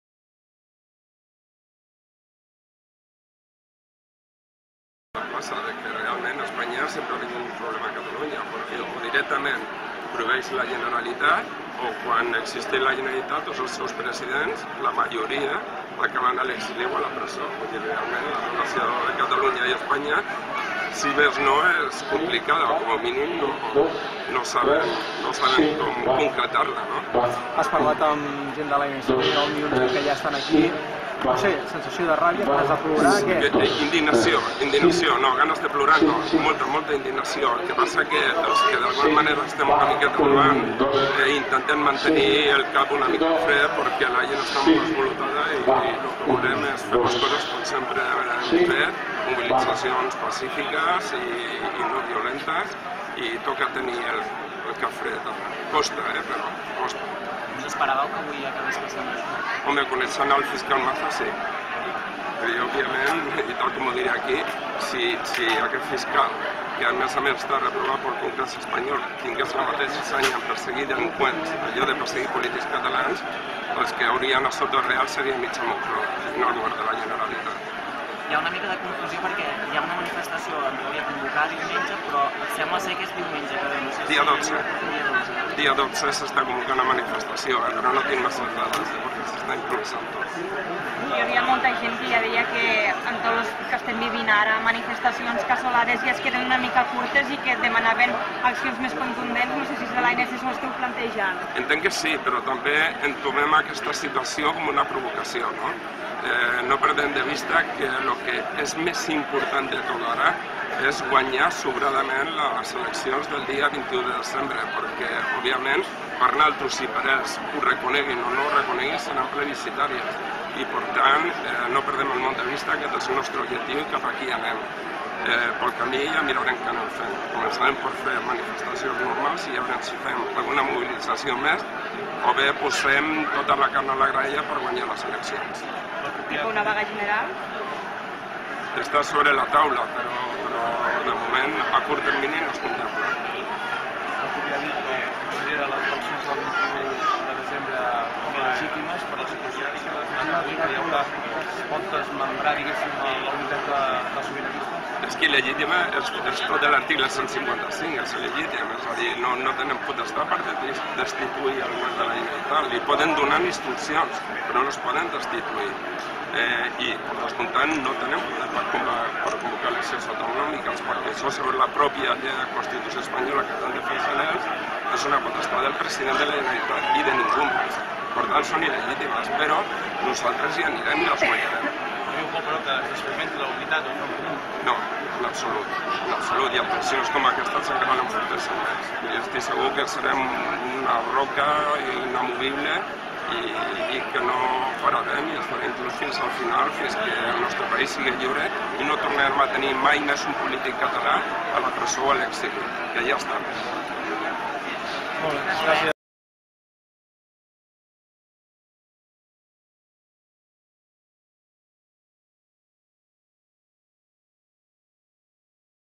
Entrevista Carles X. Gómez. Plaça Font Tarragona contra empresonament consellers CAT Puigdemont.
Carles Xavier Gómez coordinador de l’ANC de Tarragona

Més de 6.000 persones es concentren a Tarragona per rebutjar l’empresonament dels 8 membres del govern Legítim Català.

"Cap fred" i "unitat, han estat les consignes de la concentració massiva a Tarragona, en suport als empresonats. Des de l'ANC, ERC, Òmnium, la CUP, PDECat i els CDR han reclamat tenir "el cor calent" però ara el cal és fugir de provocacions i seguir amb la lluita "pacífica" al carrer. "Vam començar la revolució amb el somriure i amb el somriure l'acabarem" i per aconseguir-ho, apunten des del bloc sobiranista, cal una "candidatura unitària independentista".

Crits de "no passaran", "fora les forces d'ocupació" i Ballesteros dimissió" han estat algunes de les proclames que s'han escoltat a banda de les més entonada que ha estat "llibertat".

També han carregat contra el Fiscal General de l'Estat, José Manuel Maza, a qui li han dit que si posés la mateixa sanya a perseguir els delinqüents, "qui estaria a la presó seria la meitat de la Moncloa" i no els Jordis i gran part del Govern.

Plaça de la Font.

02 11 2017